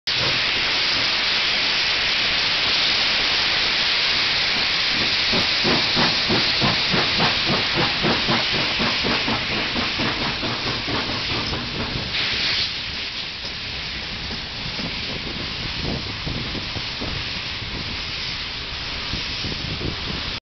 Steelwork Baotou: Steam locomotive SY1518 shunts slag wagons / Dampflok mit Schlackewagen. DCN000810
The Chinese Mikado steam locomotive SY1518 shunts several slag wagons in the Baotou Steelworks, a district-free city in the Inner Mongolia Autonomous Region of the People's Republic of China at the Huang He (Yellow River). In the railway tracks behind the steam locomotive are parked identical cars. Right next to me stops the locomotive, for me the stop of the film recording. The wagons are uncoupled and keep staying there in the track. The steam locomotive itself continues its drive solo, which you can see under the code DCN000815 or Youtube-Id = 80hjk6Yu0D8.

Dampfzüge Winterdampf in China: Chinesische Dampflokomotive SY 1518 rangiert Schlackewagen im Stahlwerk von Baotou, eine bezirksfreie Stadt am Gelben Fluss. Direkt neben mir hält die Dampflokok an, für mich Stopp der Filmaufnahme. Die Wagen werden abgekuppelt und bleiben im Gleis stehen, Eisenbahn Dampfzug.